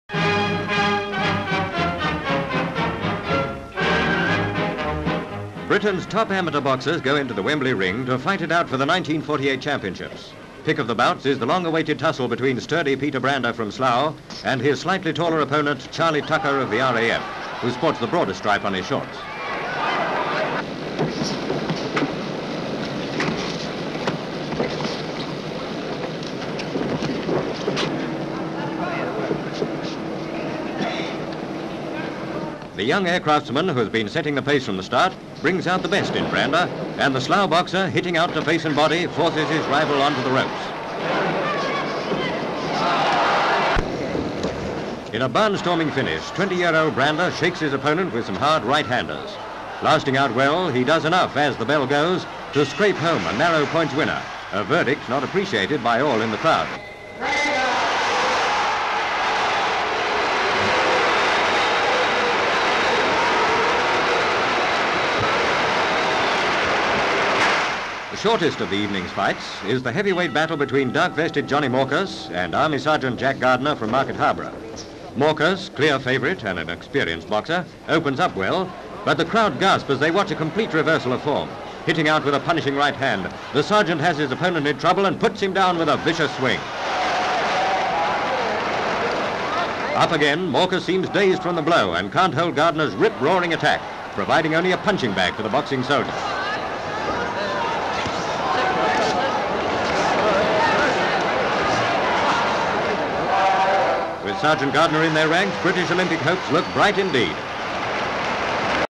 Amateurs Seek Titles (1948)
Full title reads: "AMATEURS SEEK TITLES".

Wembley, London.

Start of the boxing match between Peter Brander from Slough and Charlie Tucker at amateur boxing championship at Wembley, London. Gong - boxers come to centre of ring. Tucker lead with left blow. Tucker forces pace. Tucker connects with right hook, Brander holds. Brander hits back. Close up shot of the fighting. Toe to toe slugging. Crowd watching the match. Close up shot of Brander forcing Tucker on ropes. Close up shot of the feet. Tucker misses and Brander hits back to face and body. He forces Tucker on ropes. Crowd watching. Brander still forces pace. Close up shot of the bell. Brander wins the fight. Boxers congratulating each other. Crowd. Announcement of verdict, cheers and boos. Crowd.

Start of the heavyweight fight between Johnny Morkus and Jack Gardner. Gong and boxers move to centre. Morkus leads. Crowd watching. Gardner punching Morkus and puts him down. He rises. Gardner punching Morkus. Morkus down. End of the fight. Gardner wins.
 FILM ID:1428.19